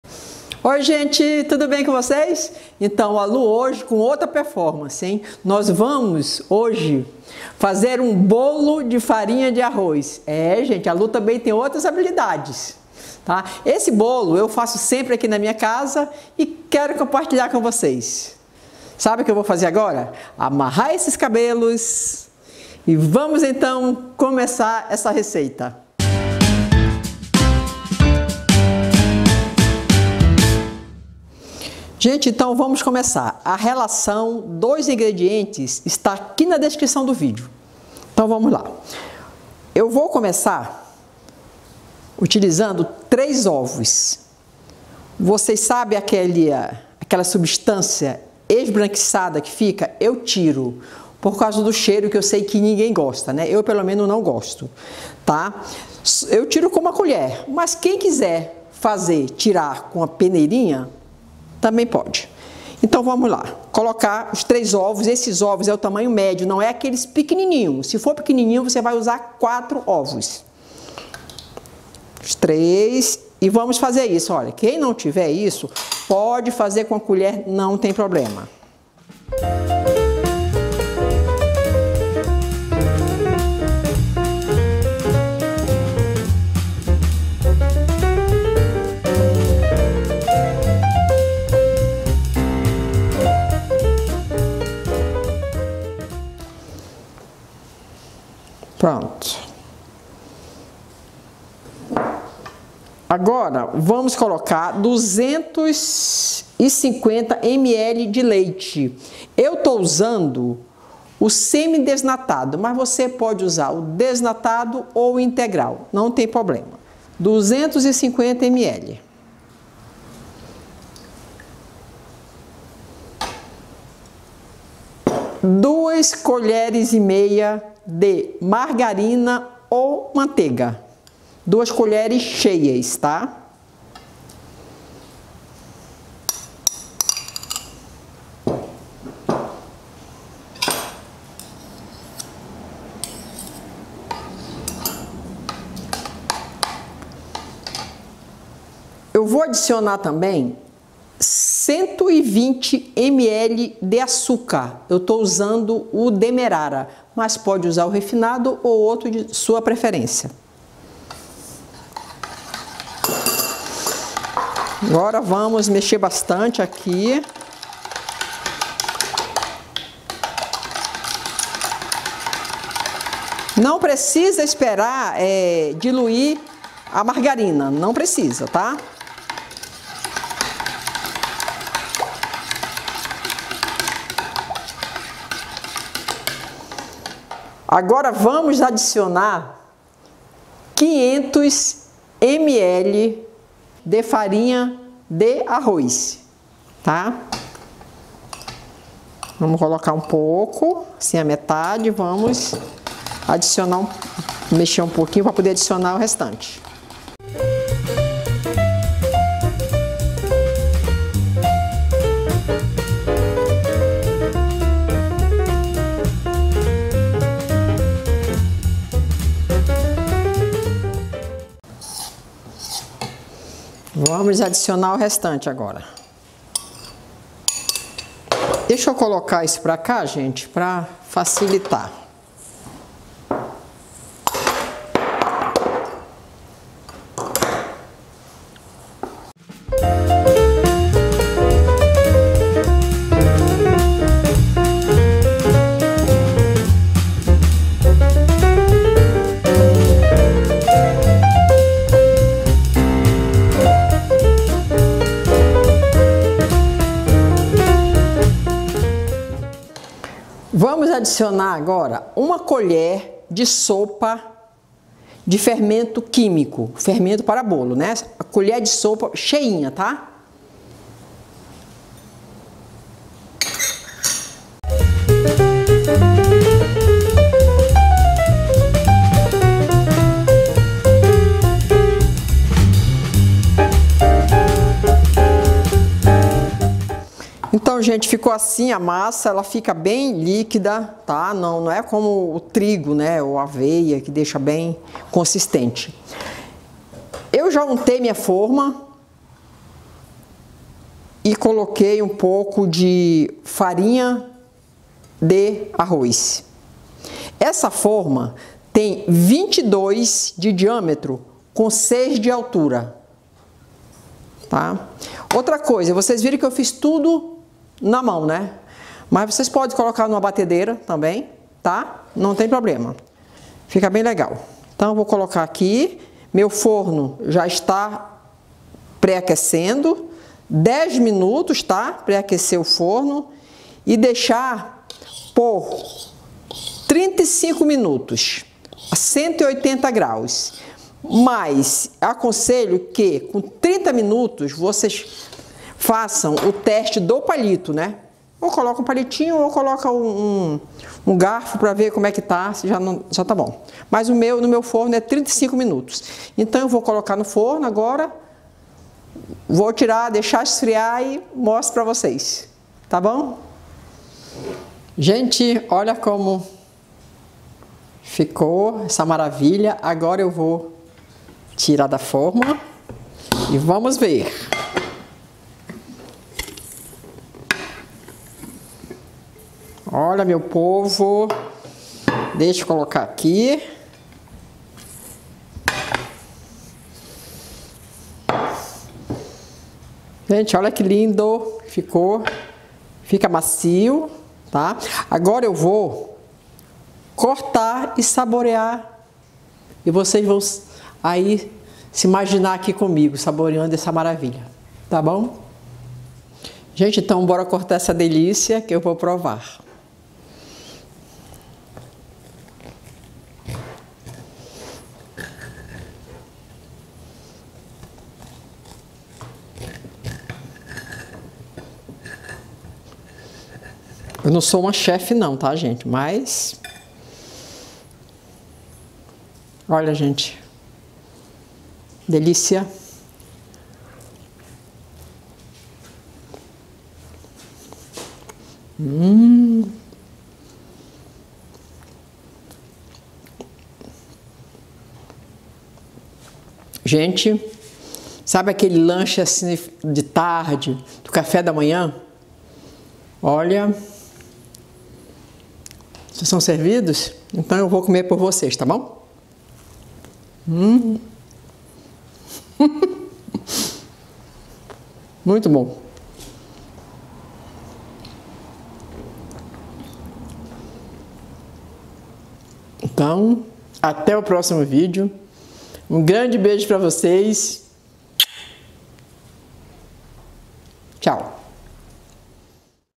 BOLO DE FARINHA DE ARROZ SEM GLÚTEN E NÃO PRECISA USAR BATEDEIRA - Lu Lampert
Olá queridas amigas e amigos!
Vamos para cozinha comigo? Hoje vamos preparar um sensacional bolo de farinha de arroz, sem glúten, fofinho, muito gostoso e muito fácil de preparar. Os ingredientes podem ser facilmente encontrados, custam pouco e a receita não precisa de batedeira e nem qualquer utensílio especial. O resultado é surpreendente, um bolo muito delicioso que pode ser servido no seu café da manhã ou lanche da tarde. Você e sua família vão adorar.

PARA PREPARAR ESTA RECEITA VOCÊ VAI PRECISAR:
3 ovos de galinha (tamanho grande)
250 ml de leite integral (pode ser semi-desnatado ou desnatado)
2 ½ colheres de sopa de manteiga ou margarina
120 ml de açúcar demerara ou de sua preferência
500 ml de farinha de arroz (2 copos de 250 ml cada)
1 colher de sopa de fermento químico

DICAS DA LU LAMPERT:
Antes de colocar a mistura na forma, unte e polvilhe um pouco de farinha para evitar que a massa grude na forma depois de cozida. Pré aqueça o forno por 10 minutos e coloque o bolo para assar na temperatura de 180° por 35 minutos. Faça o teste do palito, se ele sair limpo, retire do forno deixe esfriar, só depois de frio desenforme e bom apetite!